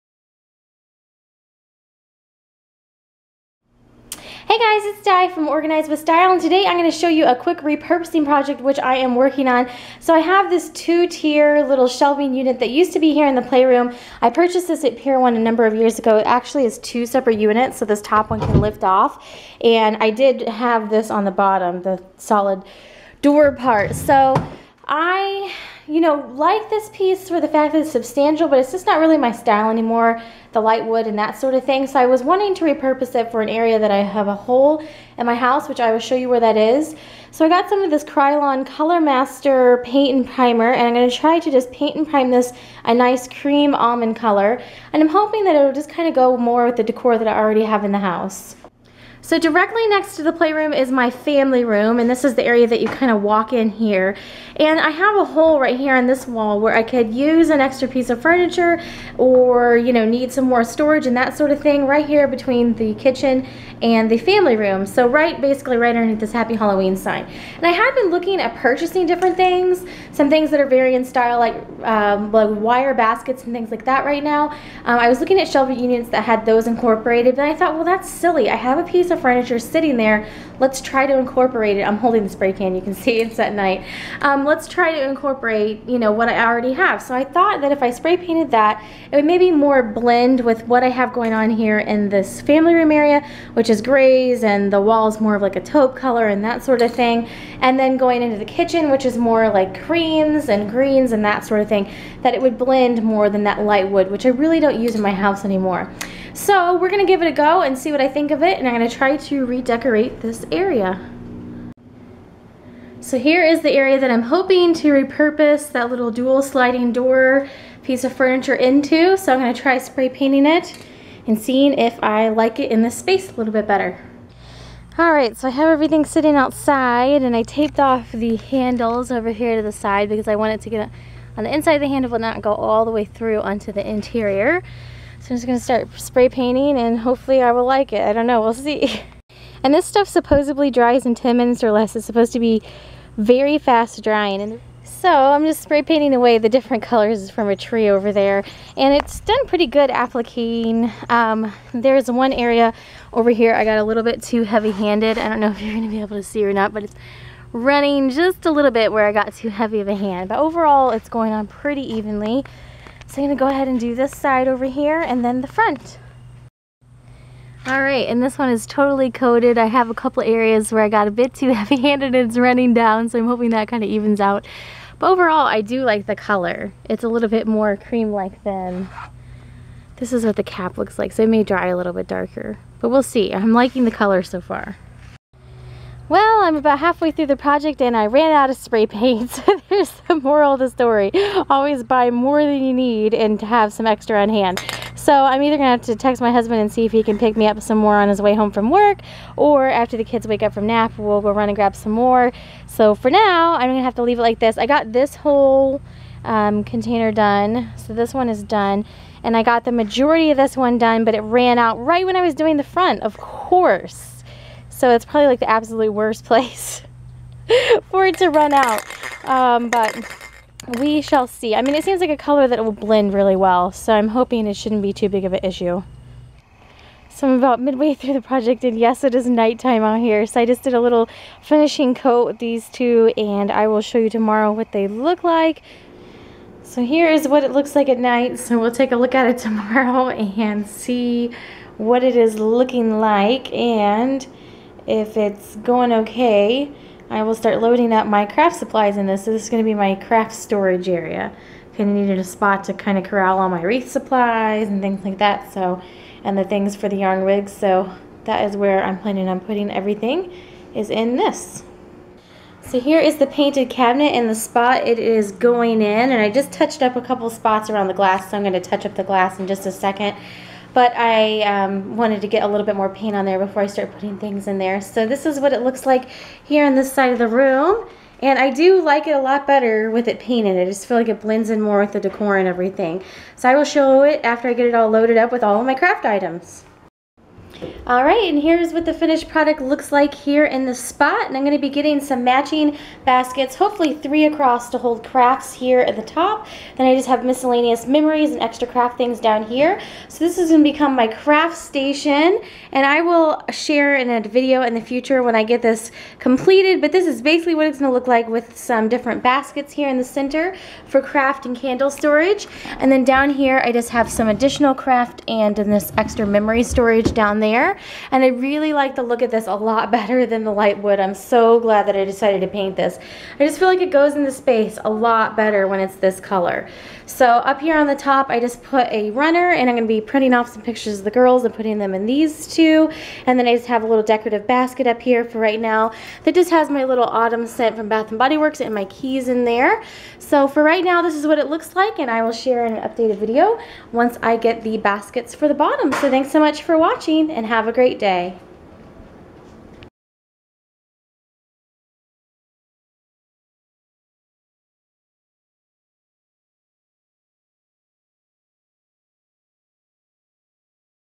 Cabinet Painting & Repurposing as a Craft Cabinet!
I repurpose the cabinet that used to hold paperwork and photo albums in the playroom/office area into a craft supplies cabinet & to fill a "hole" that we had a need for a piece of furniture in the space between our family room & kitchen.

I'm happy with how it turned out! I just need to get a few matching baskets to finish it off and add a few pictures/decorations to the top, and it will be good to go!

OrganizeMommywithStyle!